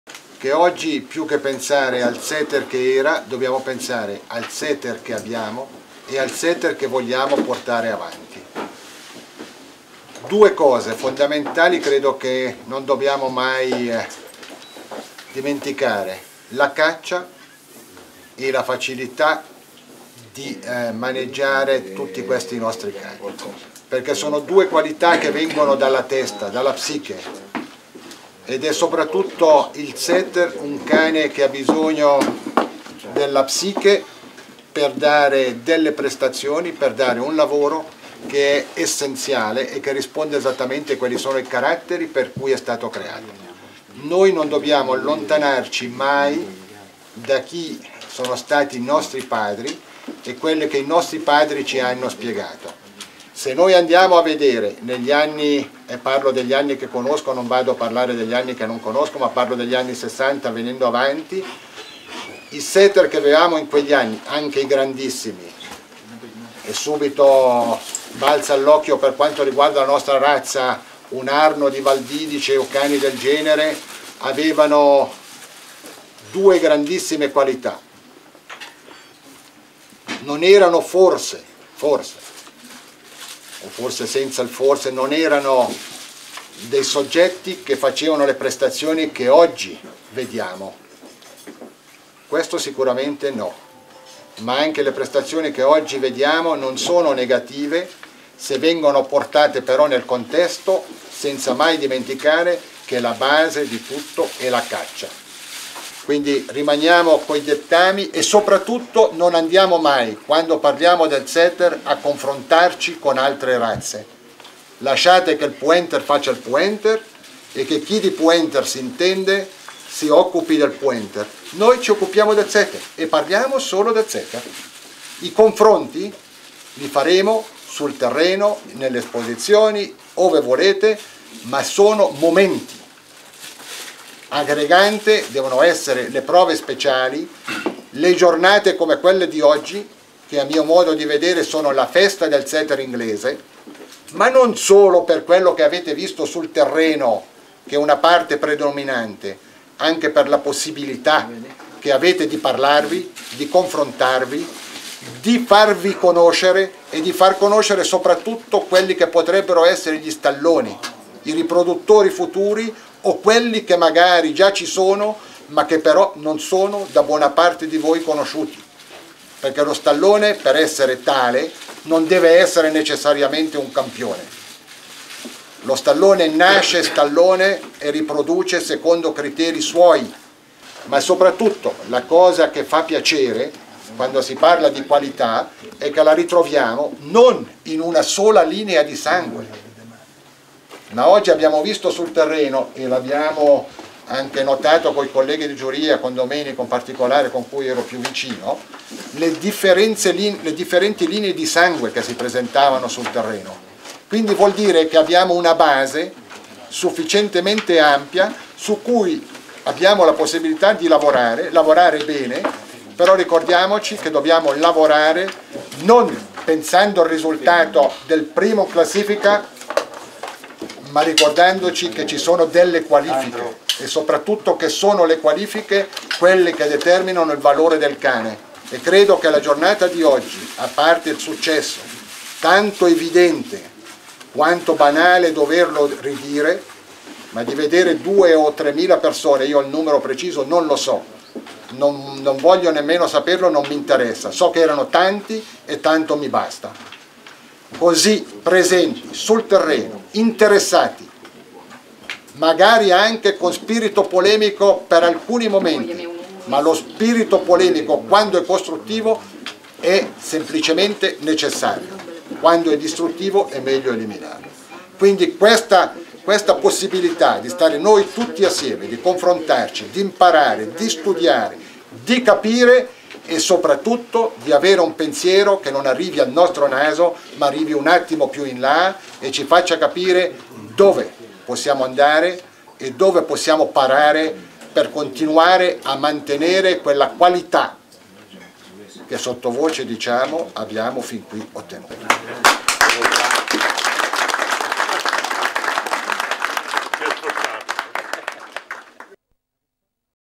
I Setter Day 2005. Pedrazzetti "Il Setter di ieri e di oggi”
Unos comentarios, para mi muy acertados, del difunto Pedrazzetti.
No olvidemos que esto fue en el 2005, hace ya 15 años, las polémicas que se dan ahora seguramente también existían hace 25 o 30 años.